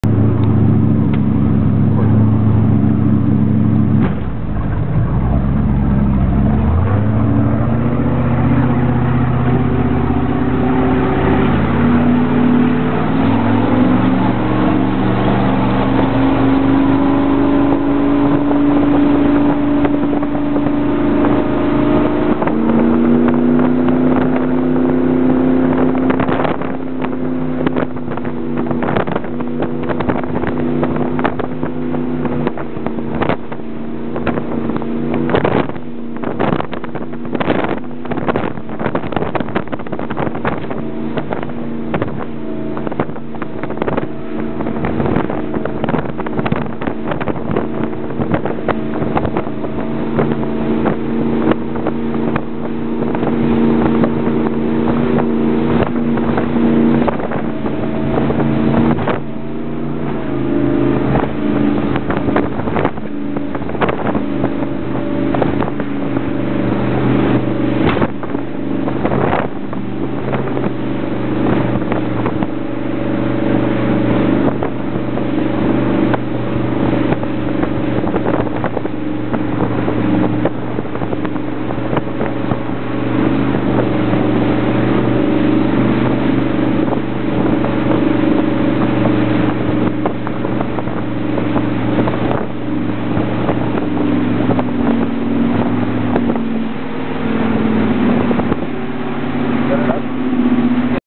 Stratford Shoals Light House - Long Island Sound
This is another light house video.  This is circling the Stratford Shoals Light House from the South side (near PJ) around the East Side out to PJ for some lunch.